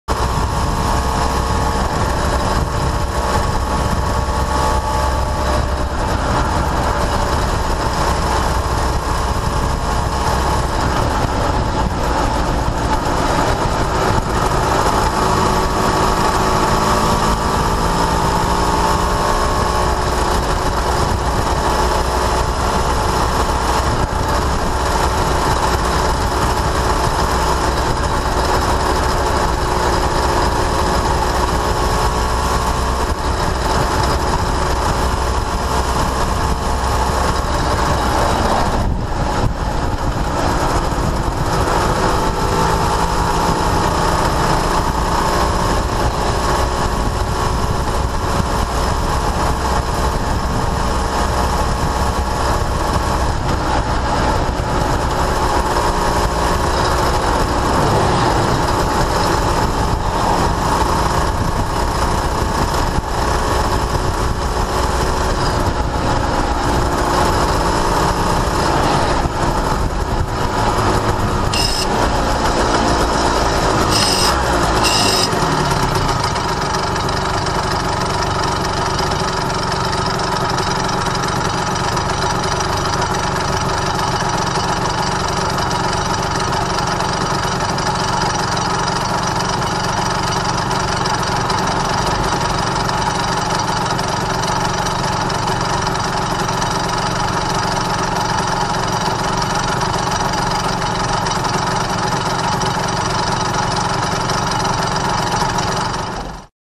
NSR250R Gopro付けてちょっと走ってみた
このぶっ壊れたような音がよいのです